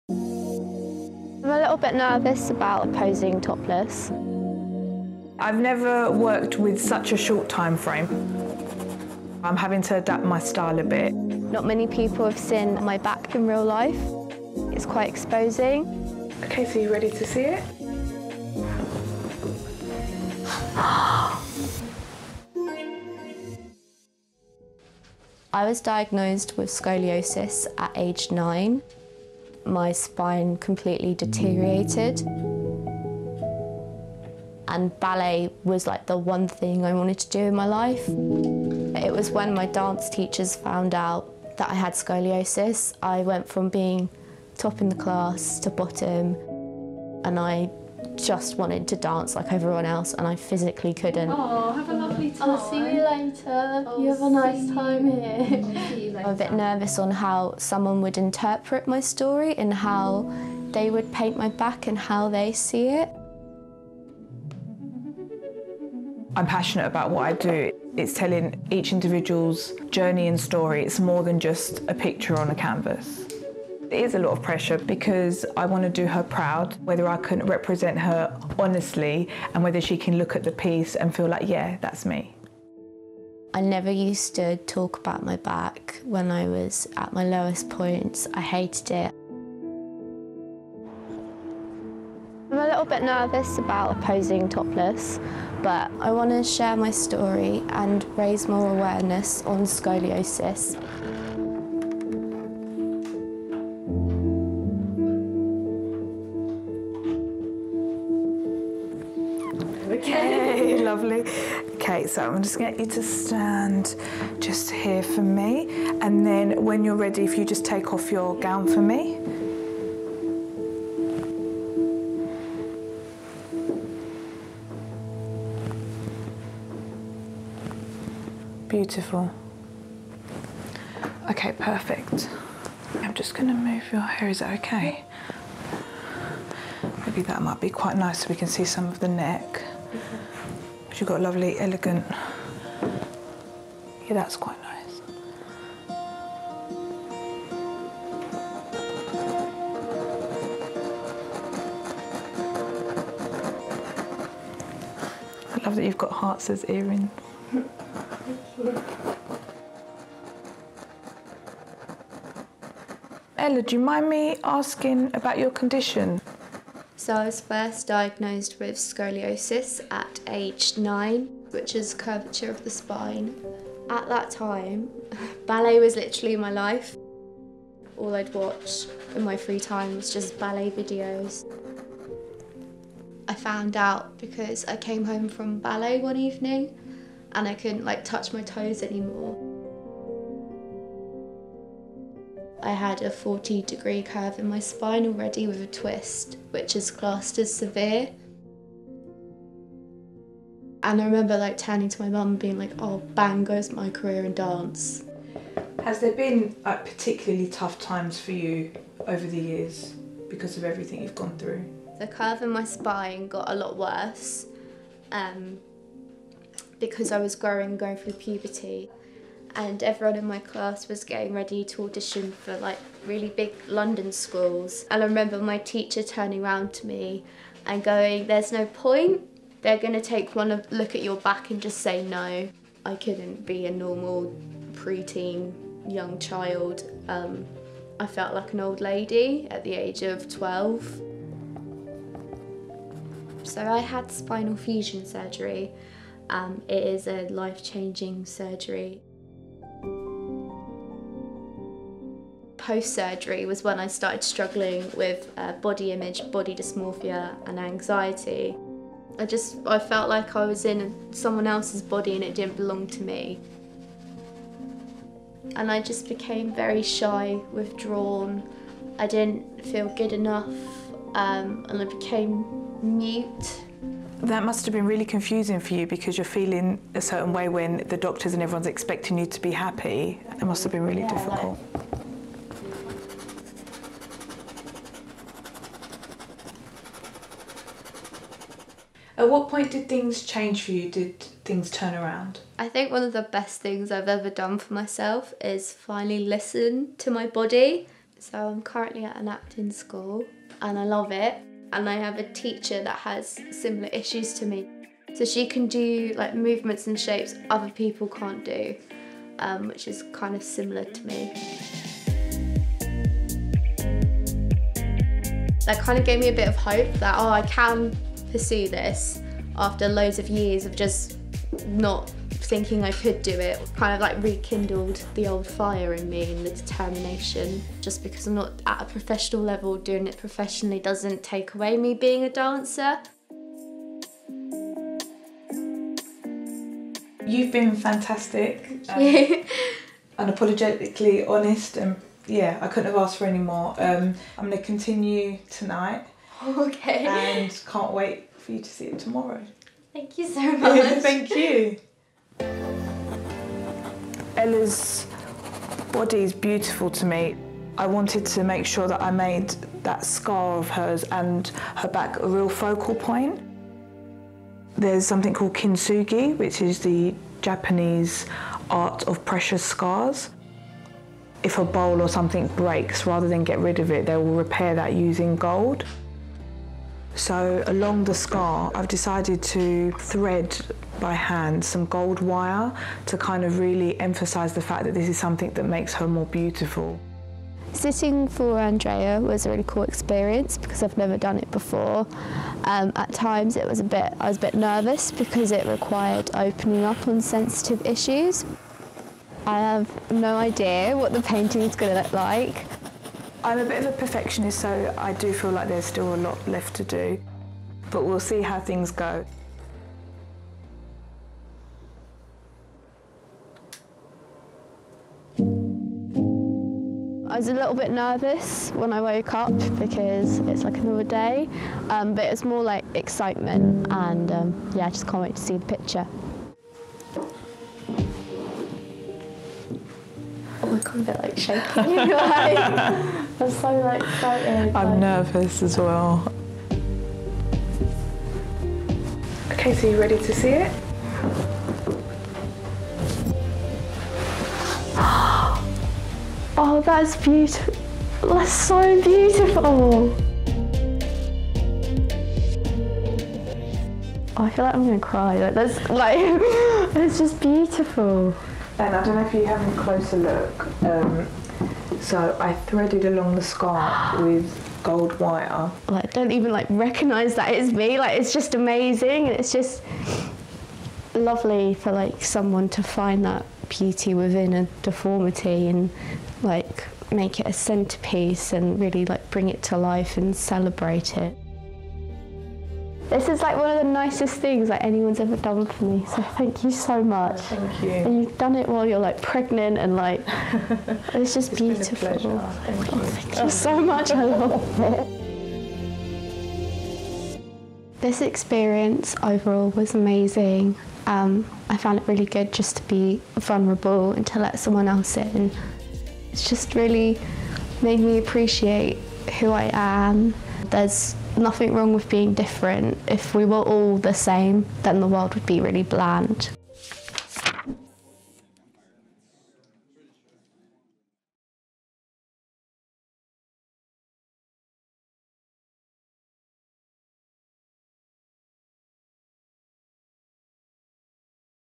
Ela Finds Beauty In Her Portrait | Flawless
Ela was diagnosed with scoliosis at a young age which caused her spine to curve and needed surgery to correct. Today she sits with artist Andrea who will paint a portrait of Ela’s scar with the hope she see’s herself differently.